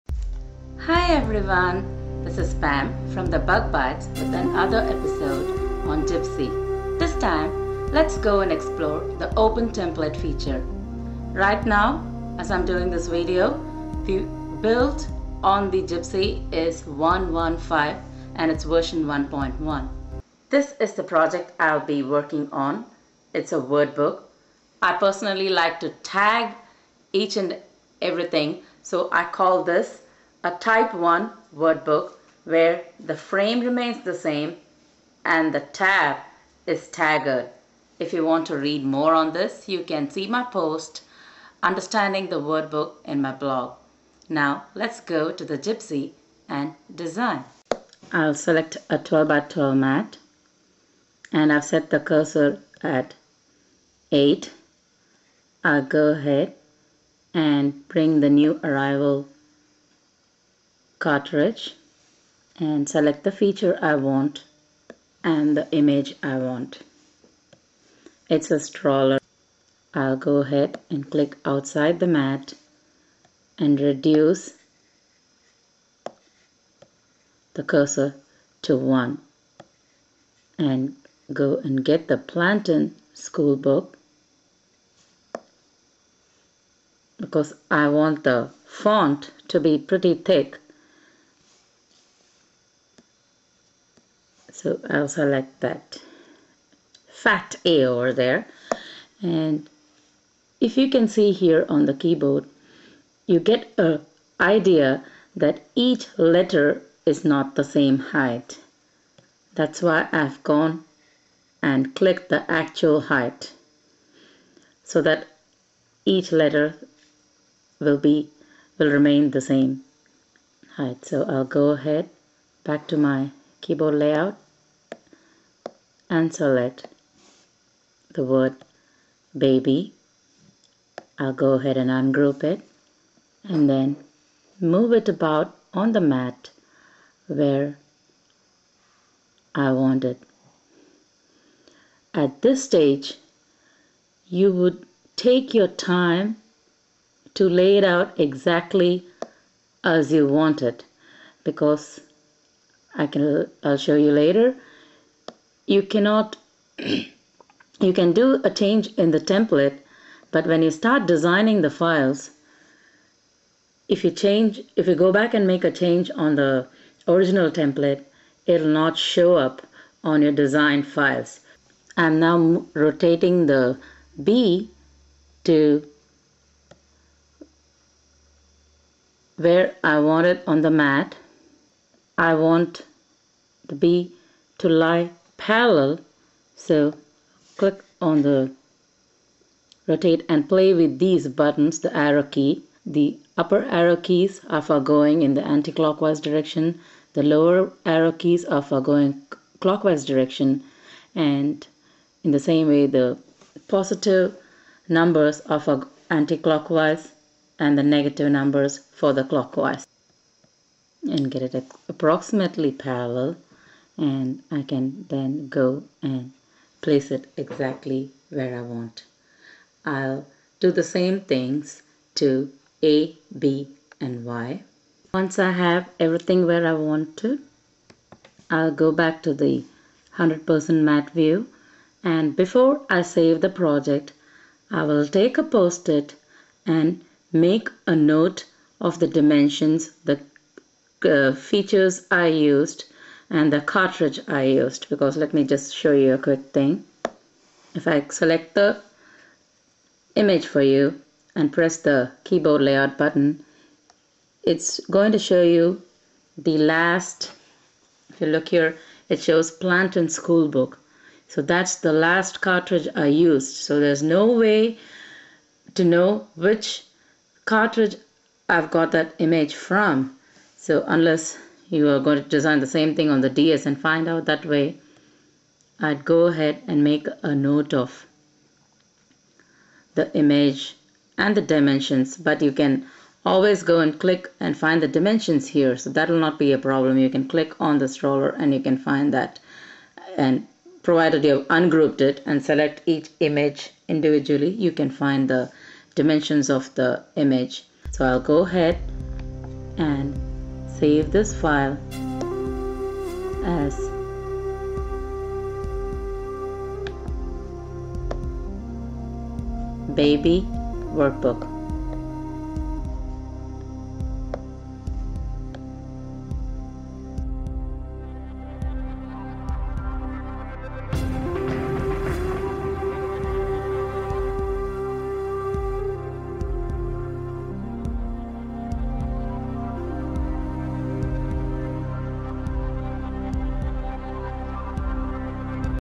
Cricut Gypsy Episode 4 - Part 1 - Open Template Feature
This is another video on the Cricut Gypsy.
This time the video covers the Open template feature. This is part 1.
Note: these videos are made before the update which got us lots of more features possible. Gypsy keeps changing with updates.